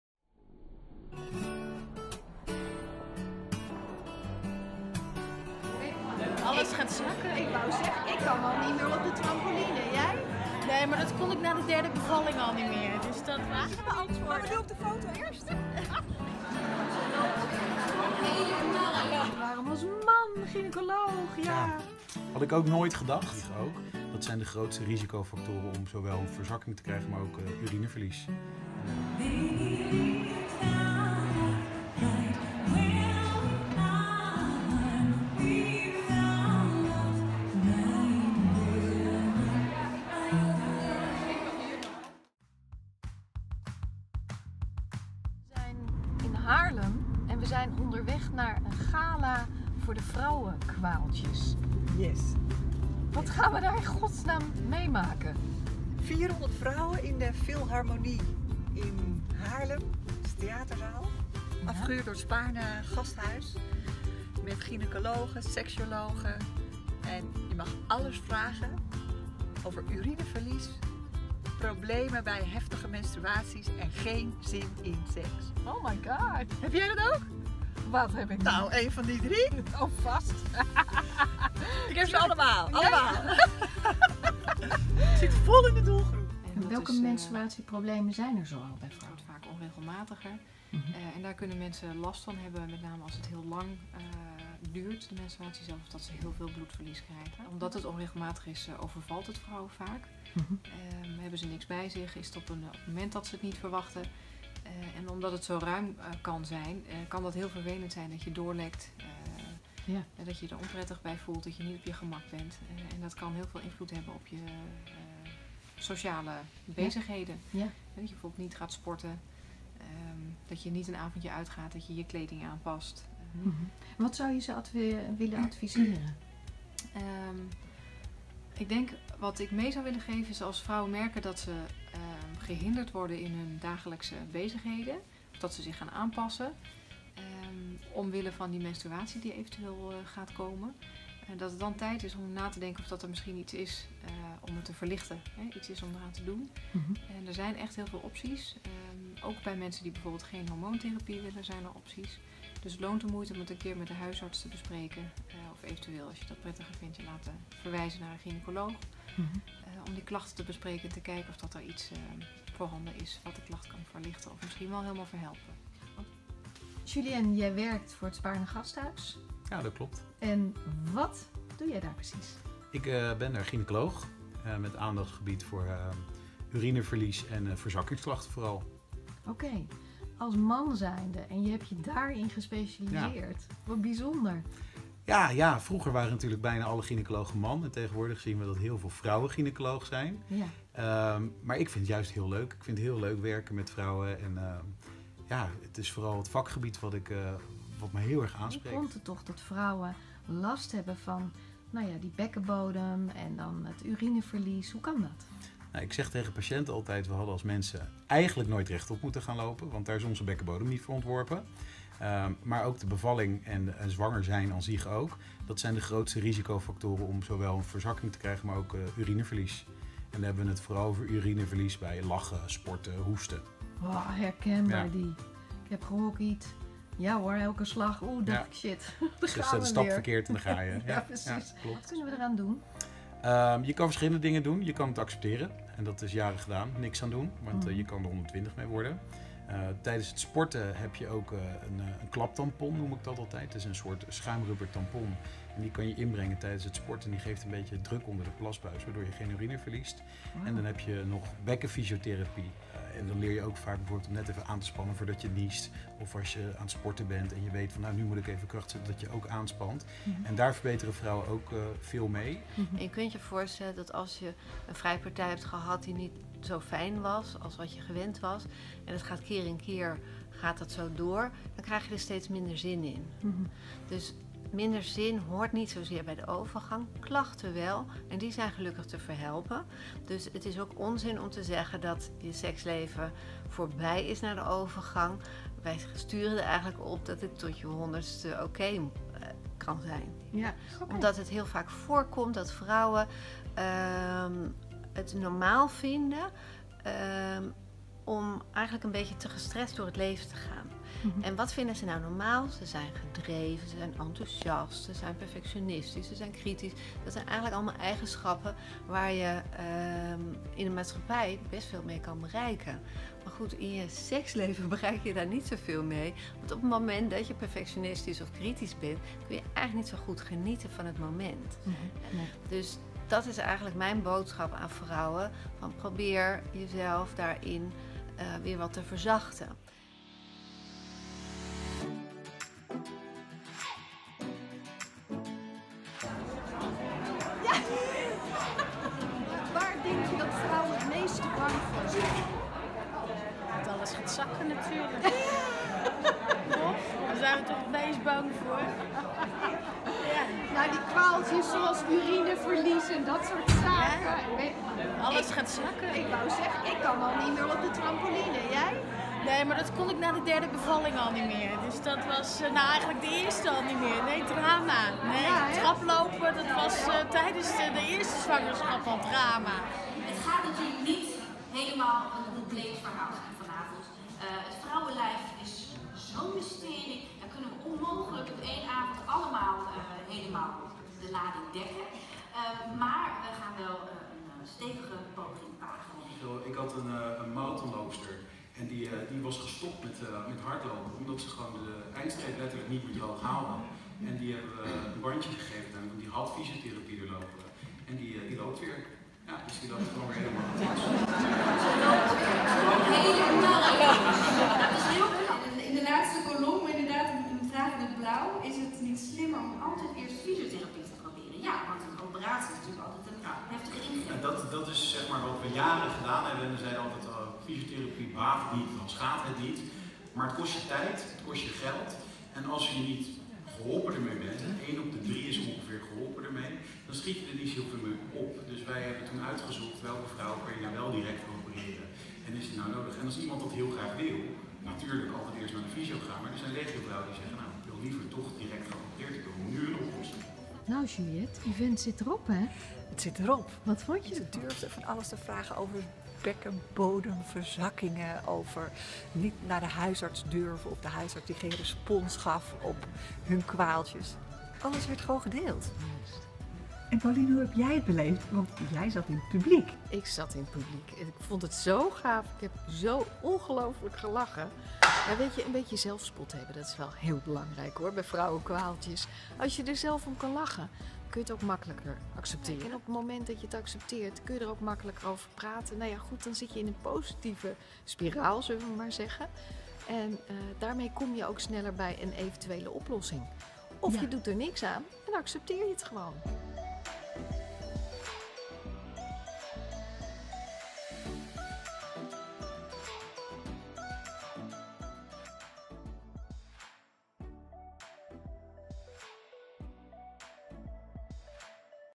PienTV - bij het Gala van de Vrouwenkwaaltjes
PienTV is voor vrouwen van deze tijd. Vrouwen die werk/carrière en gezin combineren. Vrouwenkwaaltjes hebben we allemaal, daar hoeven we ons vooral niet voor te schamen. Om te zien hoe andere vrouwen hierover denken en te leren van wat specialisten zeggen, bezocht Pien samen met Juliette Grimbergen het Gala van de Vrouwenkwaaltjes... een initiatief van het Spaarne Gasthuis.
Dagvoorzitter van het Gala was Sophie van den Enk en de Specialisten die Pien & Juul op het Gala hebben gesproken zijn:
Mirjam Zandstra; Gyneacoloog
Julien Labrie: Gyneacolooog-bekkenbodem specialist
Ingeborg Timmerman; Psycholoog, Seksuoloog